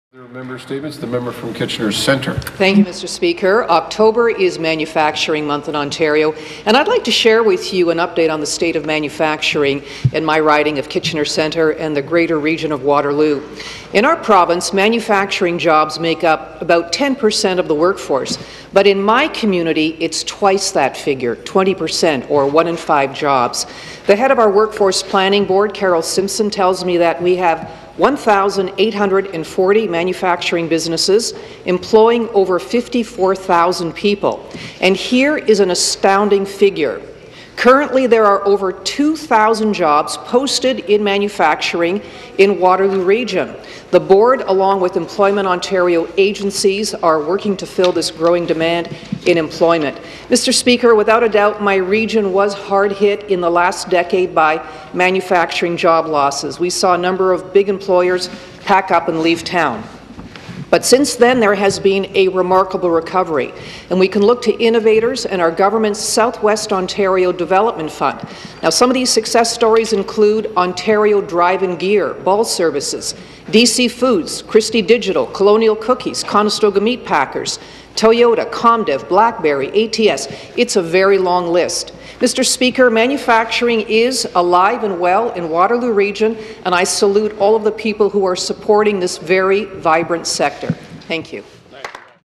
Daiene Vernile Statement re Manufacturing in Waterloo Region 10 29 15
Daiene Vernile’s Member Statement regarding "Manufacturing in Waterloo Region” on Oct. 29, 2015.